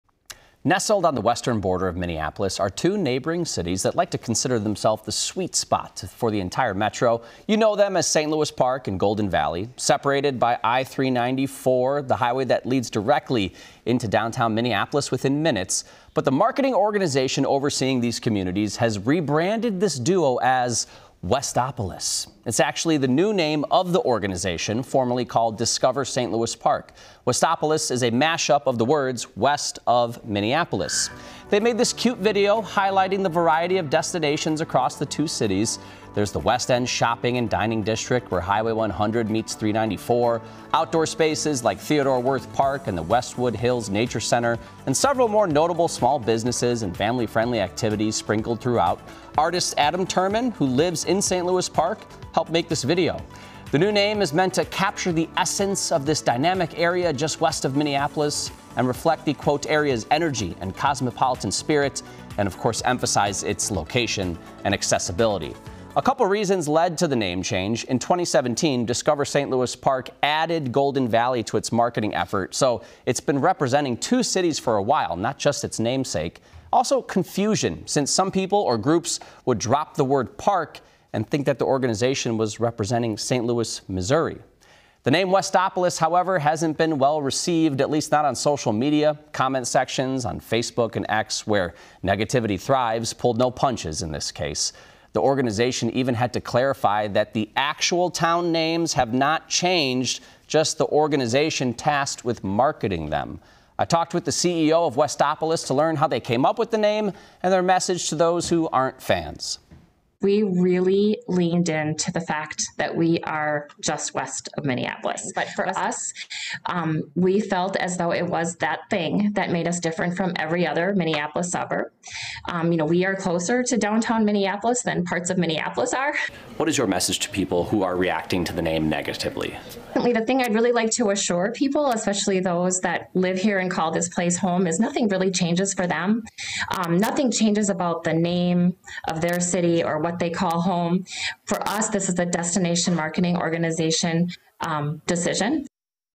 St. Louis Park, Golden Valley marketing team rebrands as “Westopolis”
Nestled on the western border of Minneapolis are two neighboring cities that like to consider themselves the sweet spot for the entire metro.